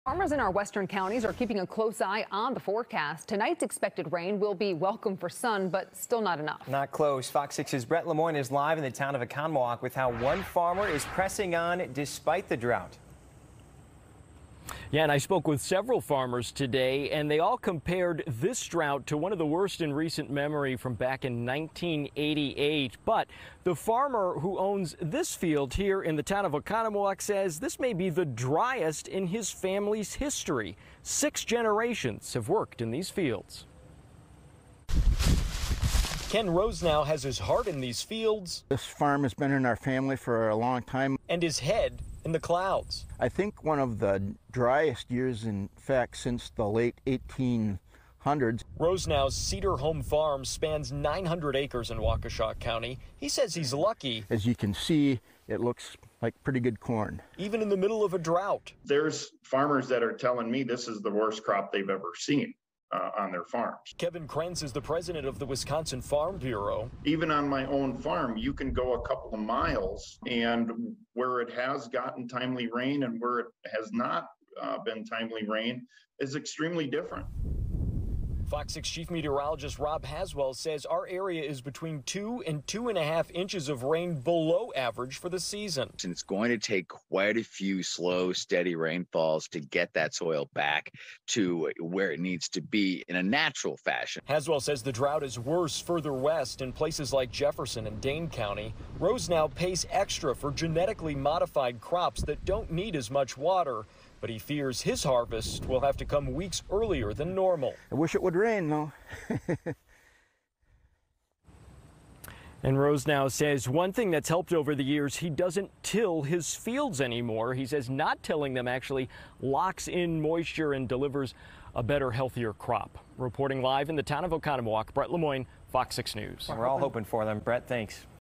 Wisconsin drought: Farmers pressing on | FOX6 News Milwaukee
Farmers in parts of southeast Wisconsin are keeping a close eye on the forecast, and Friday night's expected rain will be welcome – but still not enough.